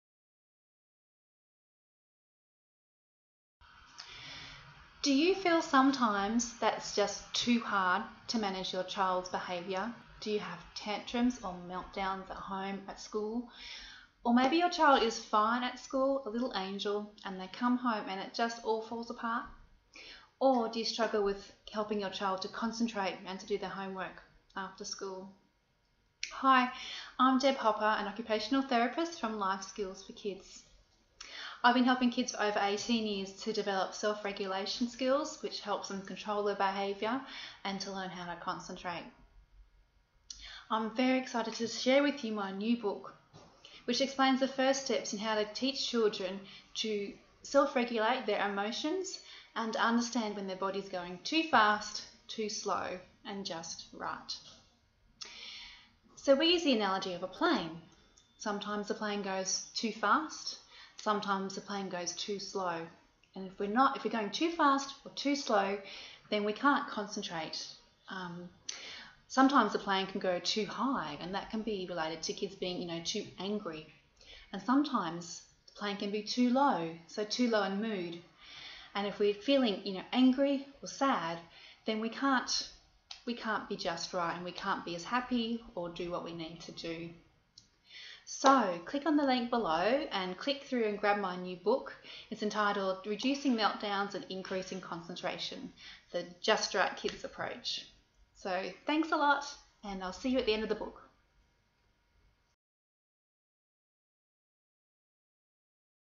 Reducing Meltdowns and Improving Concentration
Author & Occupational Therapist Deb Hopper has helped hundreds of families & classrooms to reduce tantrums, improve concentration & become happier places, through her simple, powerful & effective Just Right Kids Technique.